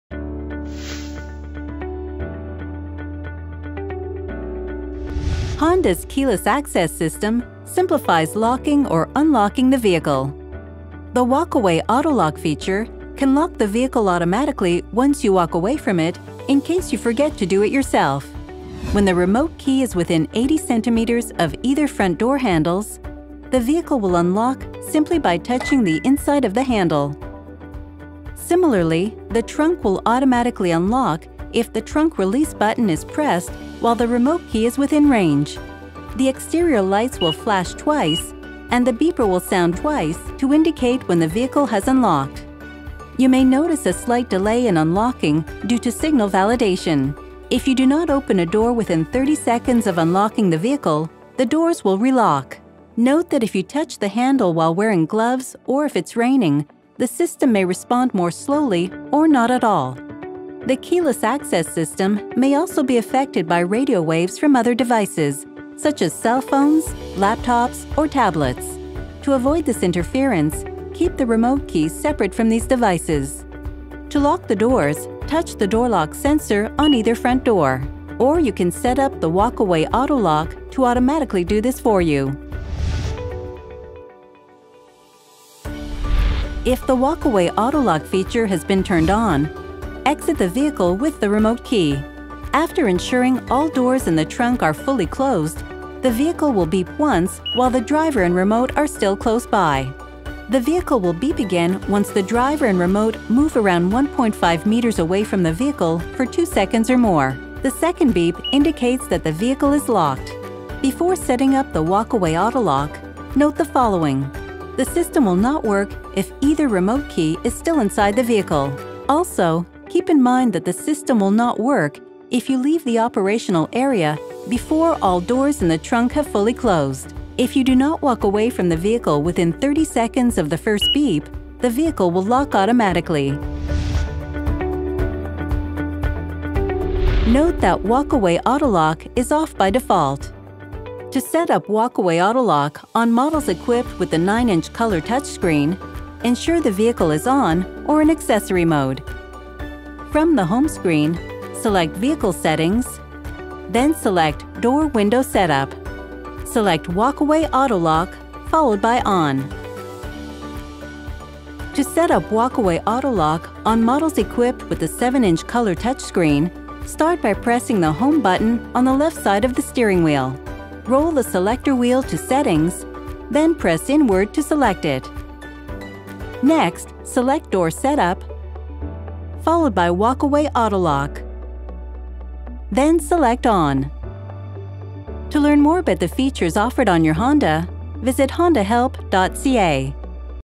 Using Your Honda’s Keyless Access System and Walk-Away Auto-Lock
Honda’s Keyless Access System streamlines locking or unlocking the vehicle while the Walk-Away Auto-Lock can handle the door-locking process automatically.